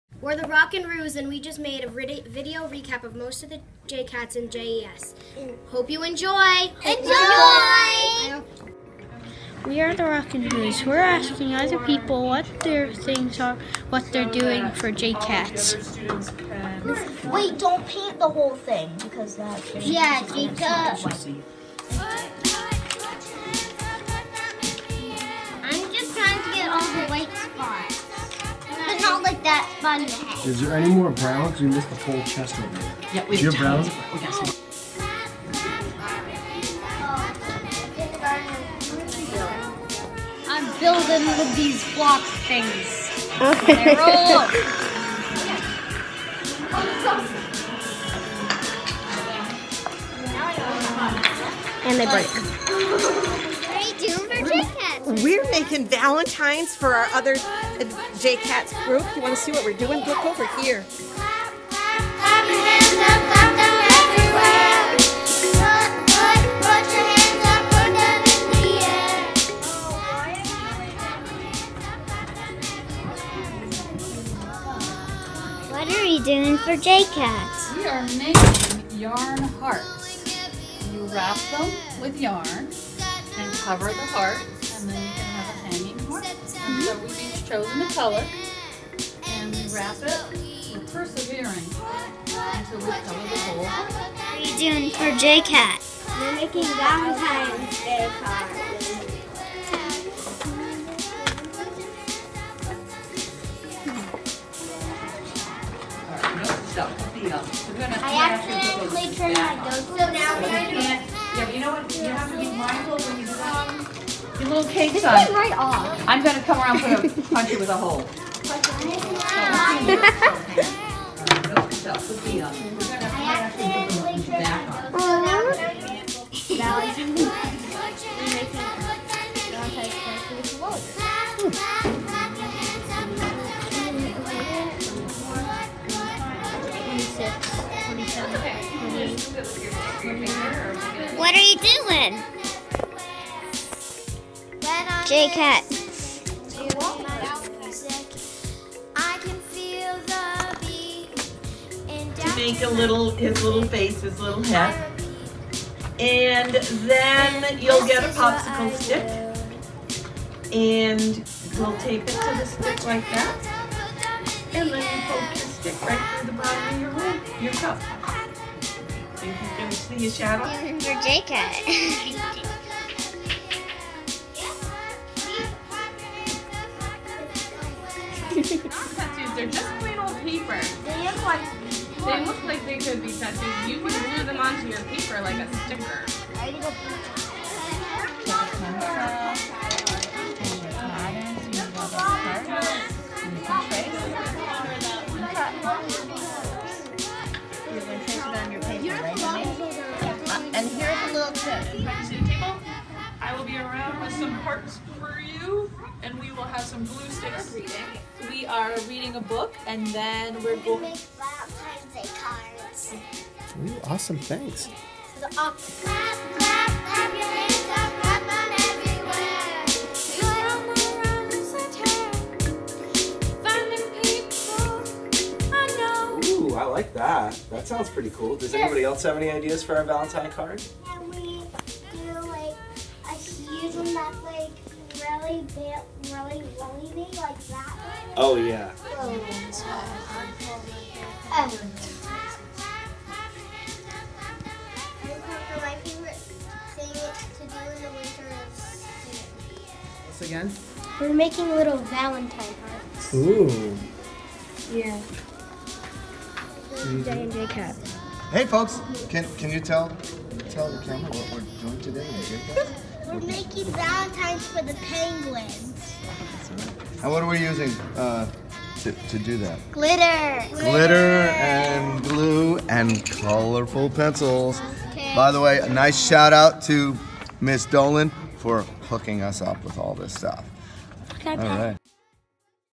A look at JES JCAT advisories
The Kangaroos spread out to record and report on JCAT activities!  Each week adults in the building host 8 students in mixed grade groupings.  Groups do crafts, build, read, do math challenges and more.  The ultimate goal is to have students meet new people and adults.